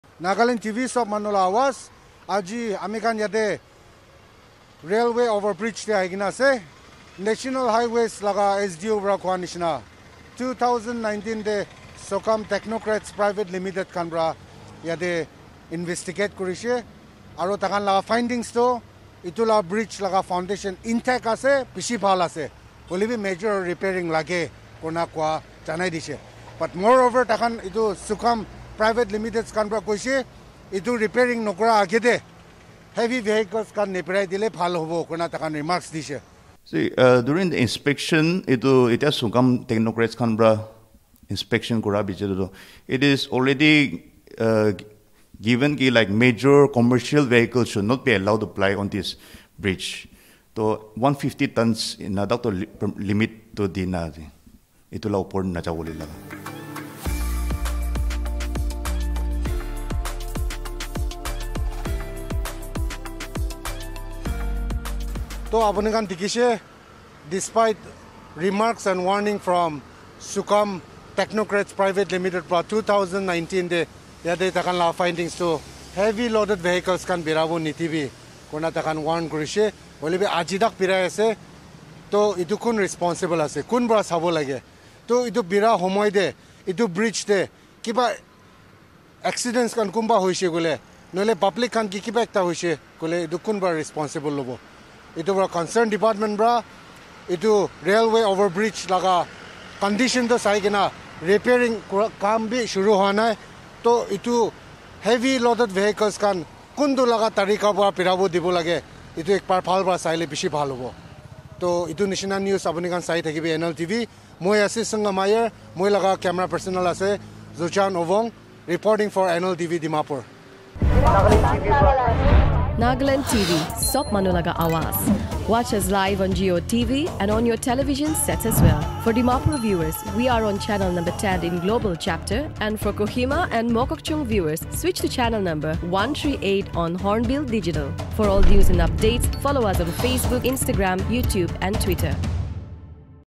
MOVEMENT HEAVY LOADED VEHICLE NEW
In 2019, Sugam Technocrats Private Limited during investigation at railway over bridge concluded that unless the bridge is repaired movement of heavy loaded trucks over the bridge were restricted to travel. However, heavy loaded trucks were seen using the bridge despite being restricted. The bridge will soon be on the verge of collapse, if measures are not taken on time